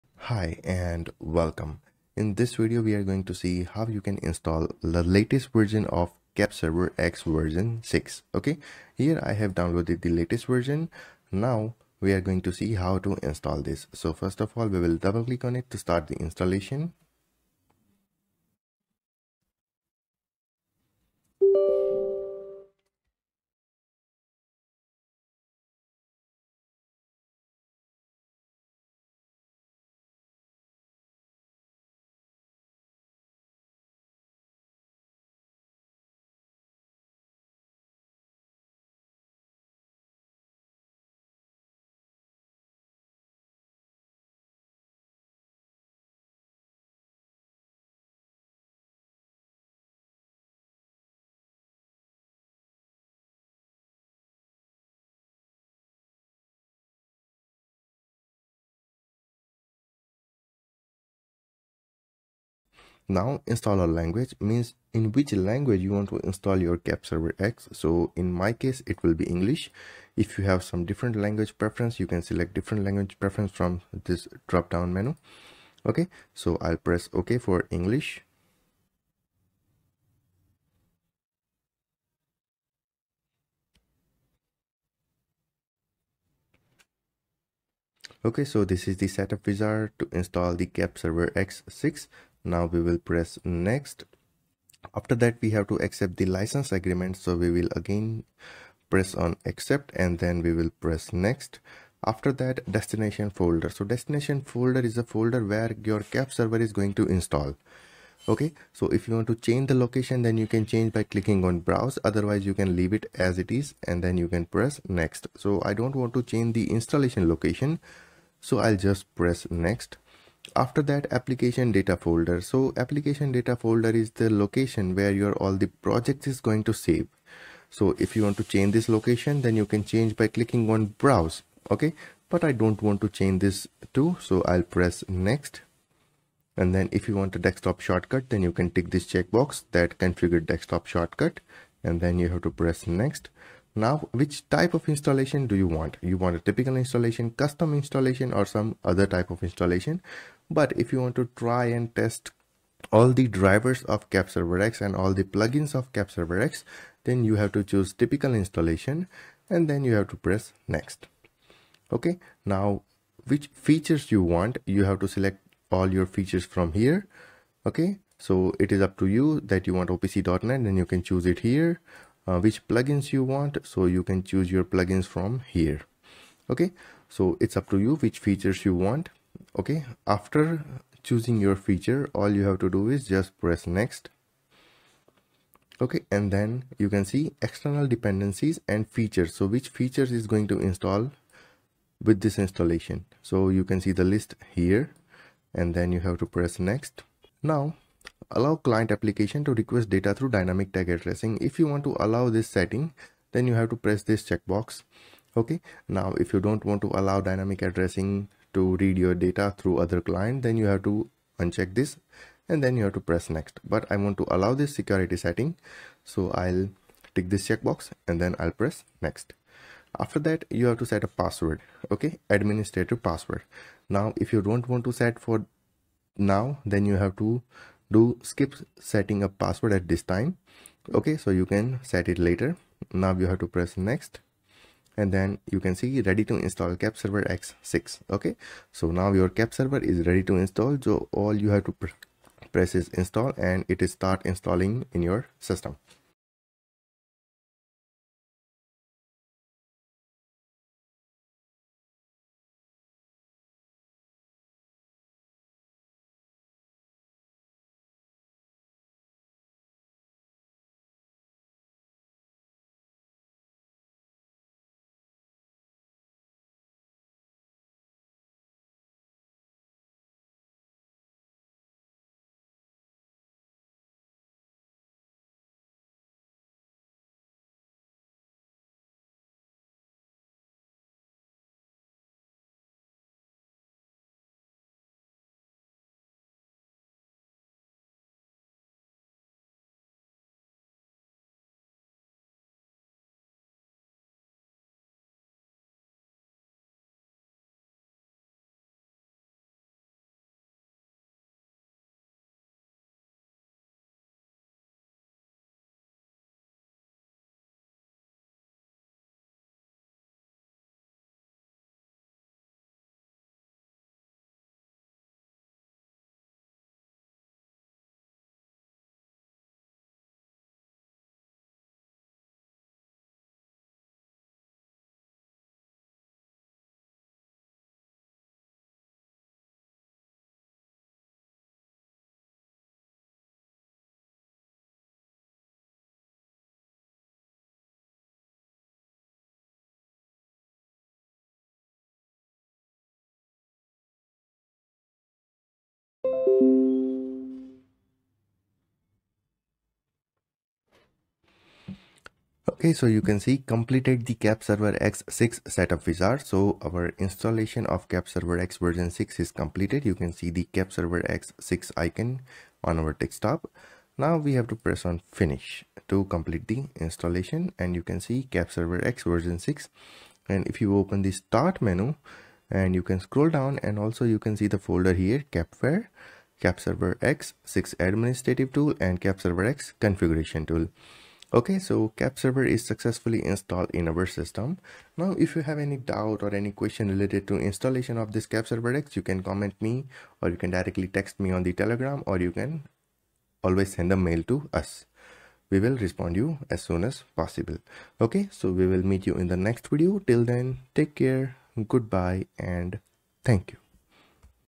KEPServerEX6 6.13.250.0 Installation Guide | IoT | PTC | IIoT | Industry 4.0
In this video, we will guide you through the installation process of KEPServerEX6 version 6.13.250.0 software. KEPServerEX6 is a server application that provides a single point of access to industrial automation data. It supports a wide range of protocols and devices, making it a versatile and powerful tool for any industrial automation system.

By the end of this video, you will have a basic understanding of how to install and configure KEPServerEX6 version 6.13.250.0 software. This will enable you to easily connect to and monitor your industrial automation data.